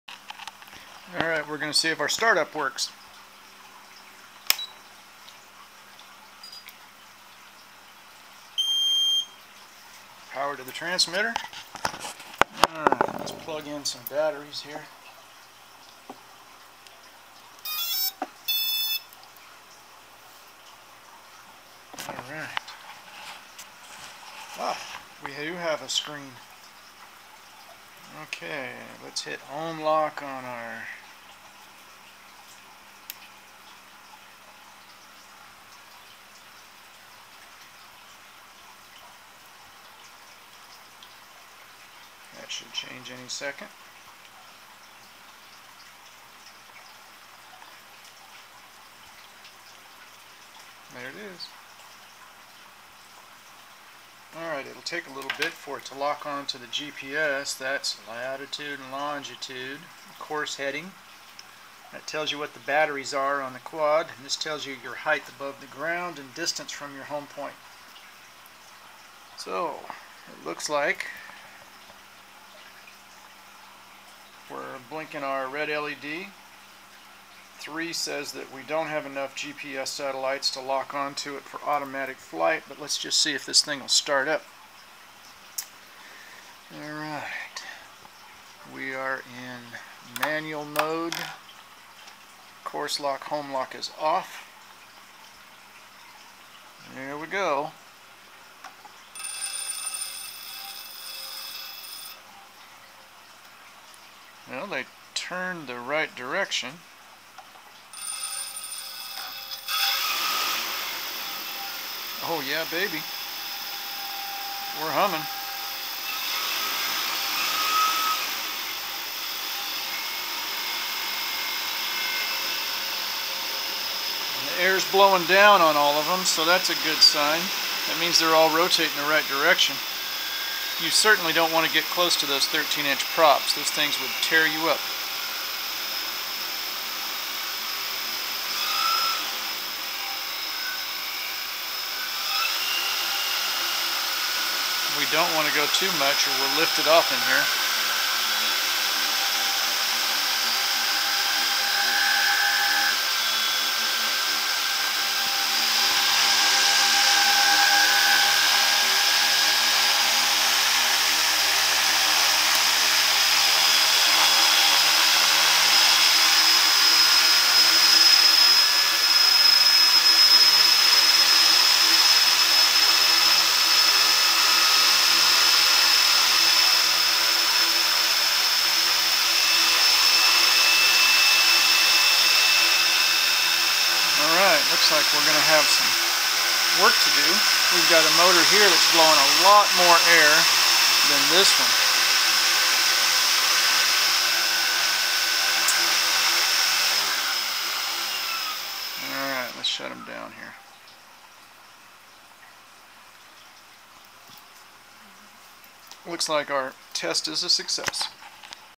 First prop and motor start up test on my Alien 680 quadcopter
Alien 680mm carbon fiber frame and landing gear (measured diag motor center to motor center) quadcopter build-up.
Into this one for about $1400.00 in parts. I already had a RC transmitter and didn't need one (if I did you could add another $300-400) Totally computer flight controlled autopilot GPS stuff here boys and girls. You still have to fly it but it's a lot easier to do now and with a FPV camera setup, it's just like you're sitting in the pilots' seat. Almost unlimited ceiling and it's not uncommon to hear of pilots taking these up several thousand feet (not supposed to and it's a little unsafe). Long gone are the days of watching the remote in the air so far away you can barely see it, and also long gone is the kink in your neck from straining to see it as well.
Anyway... Tiger motors MN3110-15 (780Kv), Simon-K 30A ESC's, NAZA-M flight controller with GPS, 960 TVL color CCD camera and a go-pro up front on the gimbal. Running 13" x 4.7 carbon fiber props, a 10,000 mAh battery for motors and a 2400 mAh battery for the video and transmitter. 5.8 Ghz x 800mW transmitter, Orange 2.4 Ghz, 8 ch receive